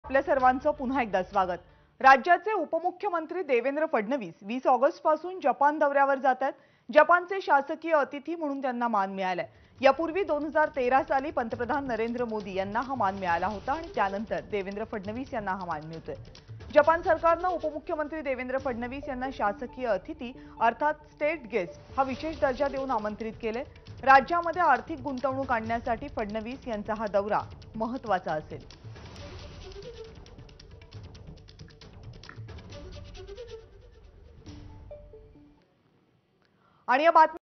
उपमुख्यमंत्री देवेंद्र फडणवीस यांचा 20-25 ऑगस्ट जपान दौरा! दौर्‍यादरम्यान 'स्टेट गेस्ट' असा सन्मान!
उपमुख्यमंत्री देवेंद्र फडणवीस यांचा 20-25 ऑगस्ट जपान दौरा! दौर्‍यादरम्यान 'स्टेट गेस्ट' असा सन्मान! | 16 ऑगस्ट 2023